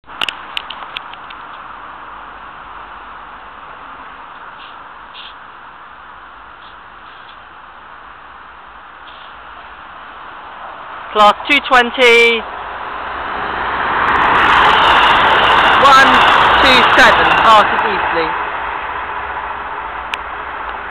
cross country class 221127 passing eastleigh
the train calls at southampton airport parkway southampton brockenhurst and
bournemouth.